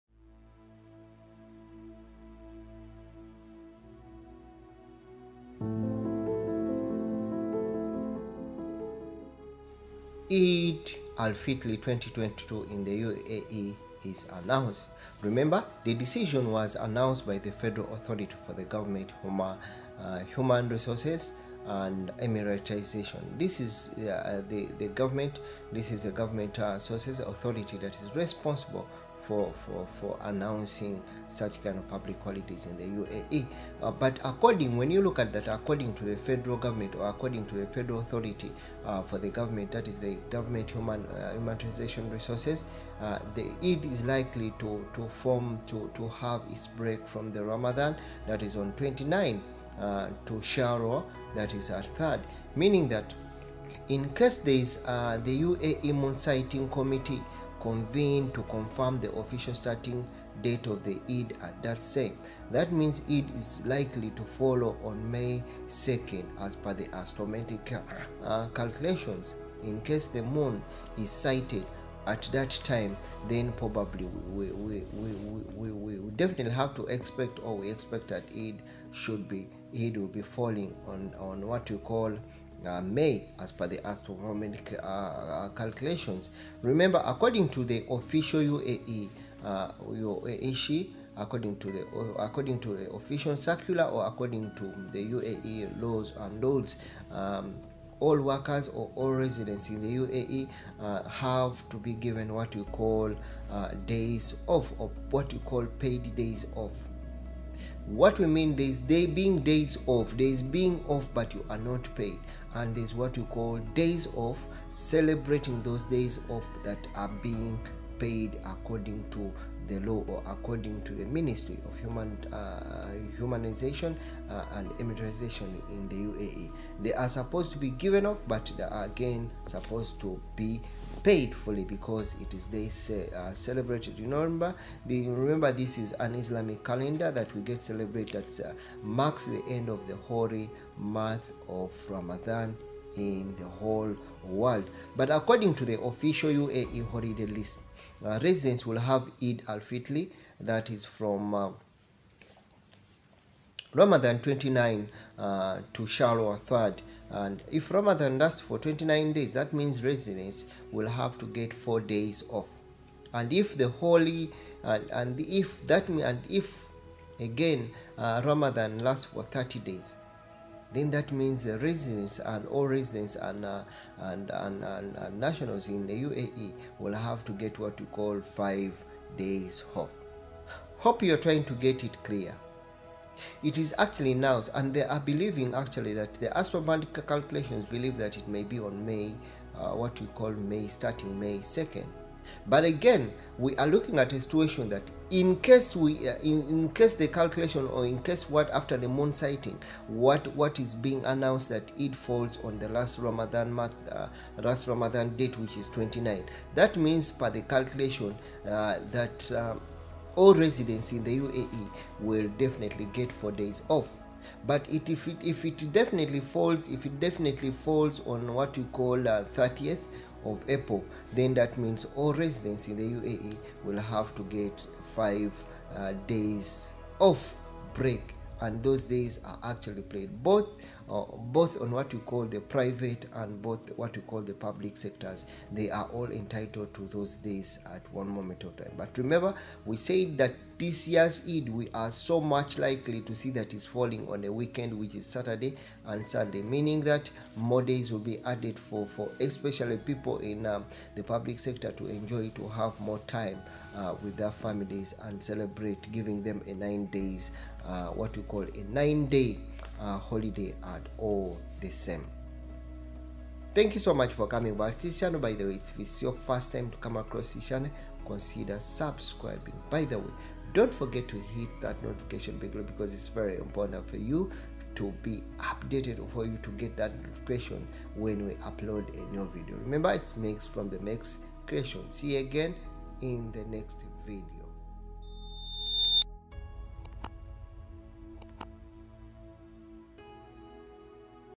UAE Eid Al- fitr 2022 ,Holidays Announced 5 days Paid Leave | 2022 UAE Holidays EID AlFITR 2022
The Eid Al Fitr holidays for the UAE federal government employees have been announced.
According to the Federal Authority For Government Human Resources, the break will be from Ramadan 29 to Shawwal 3.
The UAE moon-sighting committee will convene to confirm the official starting date for eid